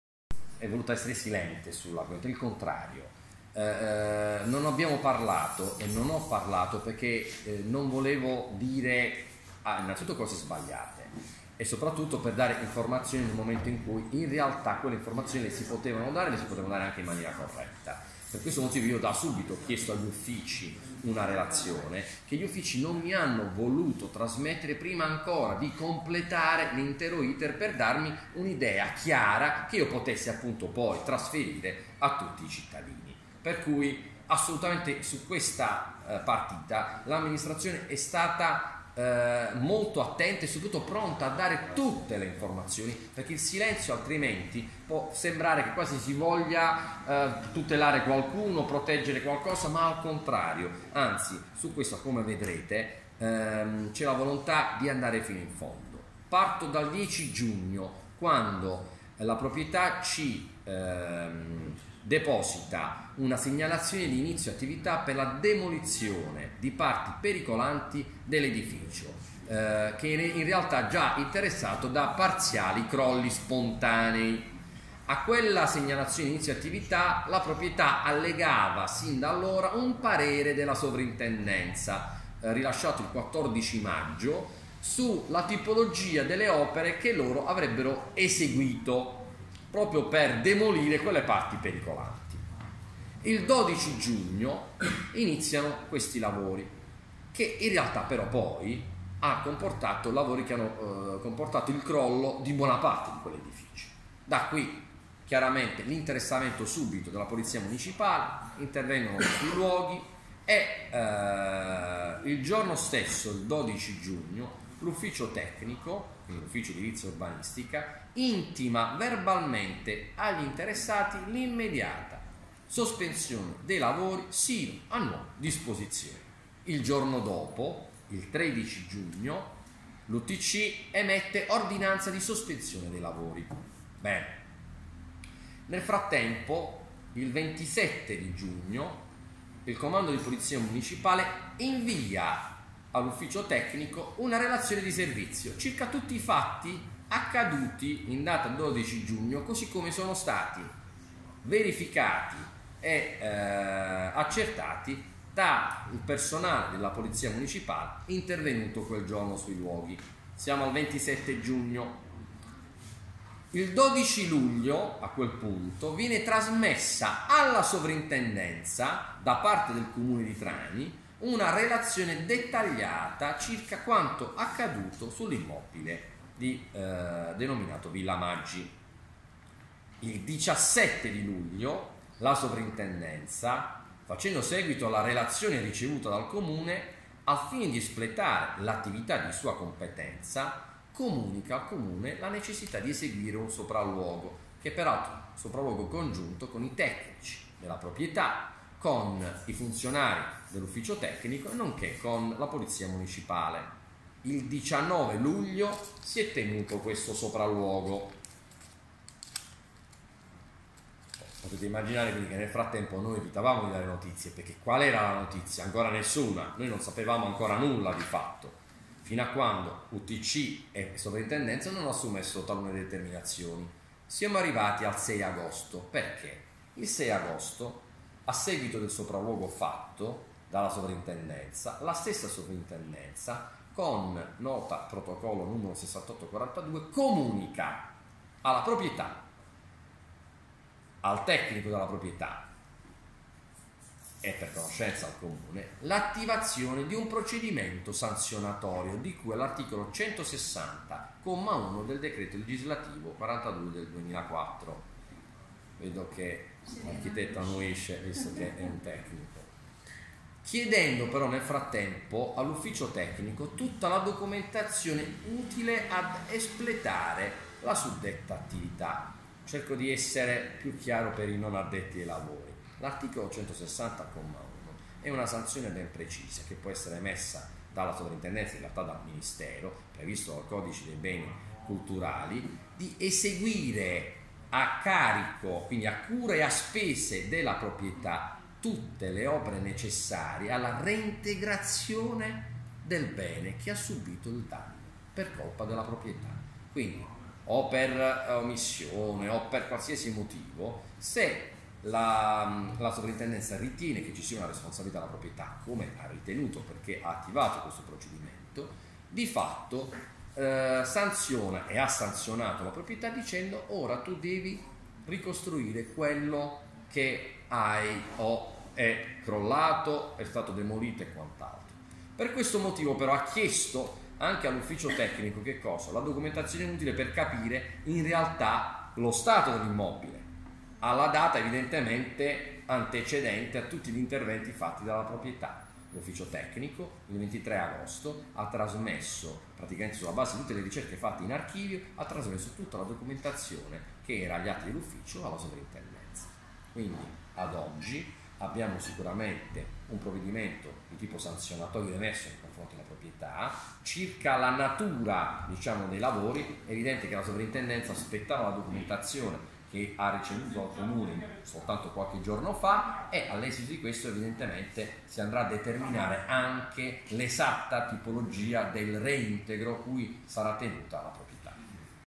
Villa Maggi il sindaco di Trani chiarisce
A margine della conferenza stampa di presentazione dell'attivazione dei parcometri a Trani, svoltasi nella sede Amet, il sindaco di Trani avvocato Amedeo Bottaro ha chiarito riguardo la vicenda Villa Maggi.